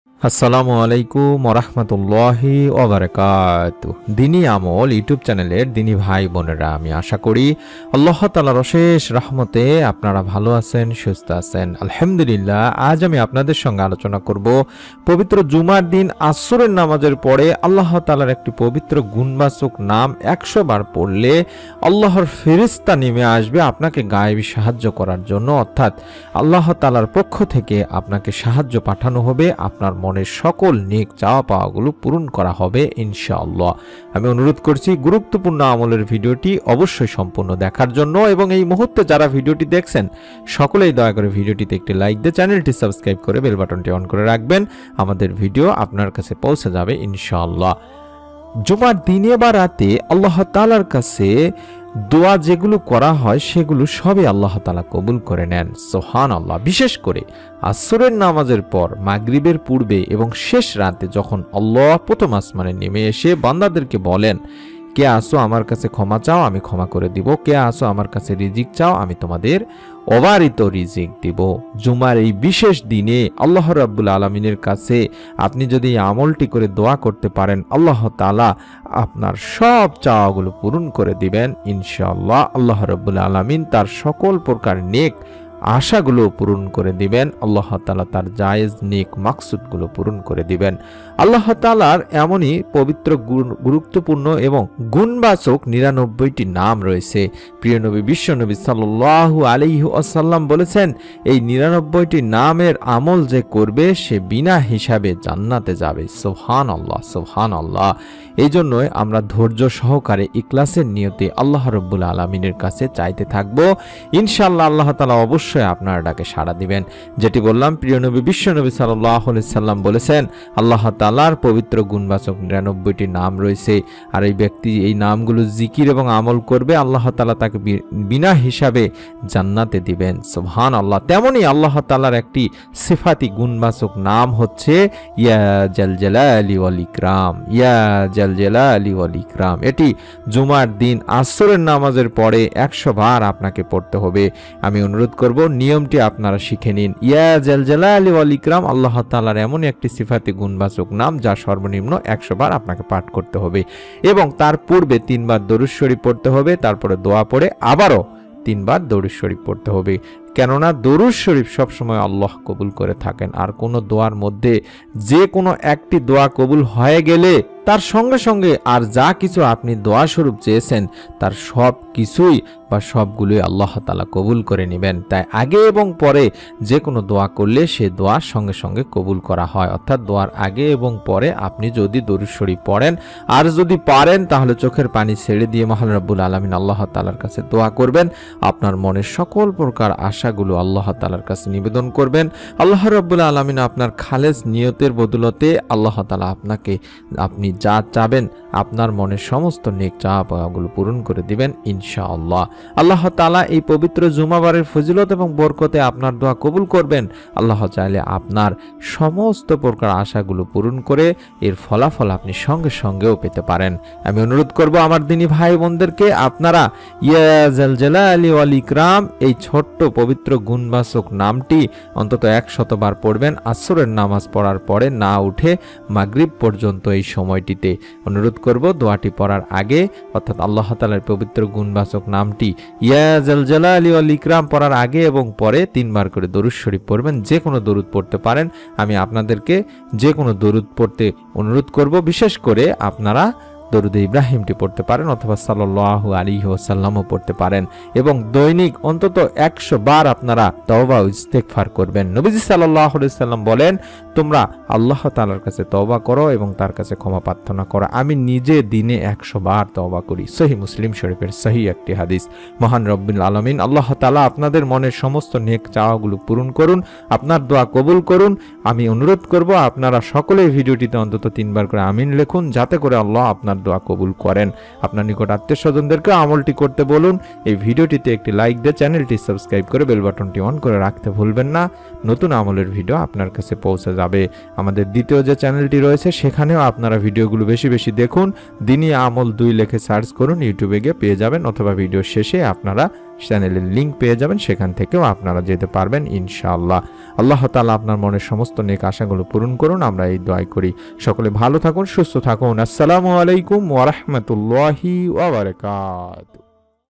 জুমার দিন আসরের পর আল্লাহে নামটি ১০০ বার পড়ুন । গায়েবী সাহায্য পাবেন । মনের সকল আশা পূরণ হবে Amol
জুমার দিন আসরের পর আল্লাহে নামটি ১০০ বার পড়ুন । গায়েবী সাহায্য পাবেন । মনের সকল আশা পূরণ হবে

জুমার দিনে যে আমলে মনের আশা পূরণ হয়ঃ

 প্রিয় নবী (সা.) বলেছেন, আল্লাহ তায়ালার গুণবাচক ৯৯টি নাম আছে। যে ব্যক্তি এই নামগুলোর জিকির ও আমল করবে আল্লাহ তায়াল তাকে বিনা হিসেবে জান্নাতে নেবেন।

আল্লাহ রাব্বুল আলামিনের সিফাতি গুণবাচক নামের একটি হলো- 'ইয়া জাল জালালি ওয়াল ইকরাম।'

জুমার দিনে বা রাতে আল্লাহ তায়ালার কাছে করা দোয়াগুলো কবুল হয়। বিশেষ করে আসরের নামাজের পর, মাগরিবের পূর্বে।

‘ইয়া জাল জালালি ওয়াল ইকরাম’ আল্লাহ তায়ালা এই সিফাতি গুণবাচক নামটি সর্বনিম্ন ১০০ বার পাঠ করবেন। এবং তার পূর্বে তিনবার দরুদ শরিফ পড়ে দোয়া পড়ে আবার তিনবার দরুদ শরিফ পড়বেন। কেননা দরুদ শরিফ সব সময় আল্লাহ কবুল করে থাকেন। আর কোনো দোয়ার মধ্যে যেকোনো একটি দোয়া কবুল হয়ে গেলে তার সঙ্গে সঙ্গে আর যা কিছু আপনি দোয়া স্বরূপ চেয়েছেন তার সবগুলোই আল্লাহ কবুল করে নেবেন। তাই আগে এবং পরে যেকোনো দোয়া করলে সে দোয়া সঙ্গে সঙ্গে কবুল হয়ে যায়।

চোখের পানি ছেড়ে দিয়ে আপনি রাব্বুল আলামিনের কাছে দোয়া করবেন আপনার মনের সব প্রকার আশাগুলো আল্লাহর কাছে নিবেদন করুন। রাব্বুল আলামিনের কাছে খালেস নিয়তে ব্যক্ত করে আল্লাহ তায়ালার কাছে সাহায্য চাইতে থাকবেন। ইনশাআল্লাহ! আল্লাহ তায়ালা এই জুমা বারের ফজিলত এবং বরকতে আপনার দোয়া কবুল করবেন। আল্লাহ চাইলে আপনার সব প্রকার আশা পূরণ করে দেবেন। ইনশাআল্লাহ!

মহান আল্লাহ আমাদের সকলকে প্রতি জুমার দিন এই আমলটি বেশি বেশি করার তাওফিক দিন । আমিন ।